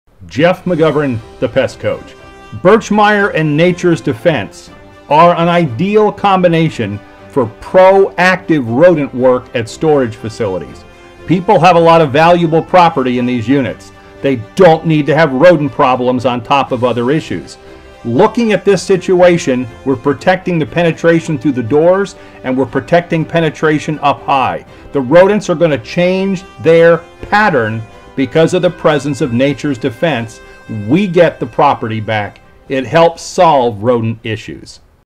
Birchmeier & Nature's Defense 10 25 2023
Combining two product lines that together deal with rodents, Birchmeier and Nature's Defense do it well.  Storage facilities can have massive rodent issues.  Units are not easily accessed on a regular basis and frankly what stored in them is sometimes unimaginable.  Nature's Defense will change the pattern of rodents at the site.  We get back the structures and keep them rodent free.  Proactive use of the product ensures any potential introductions never become an infestation.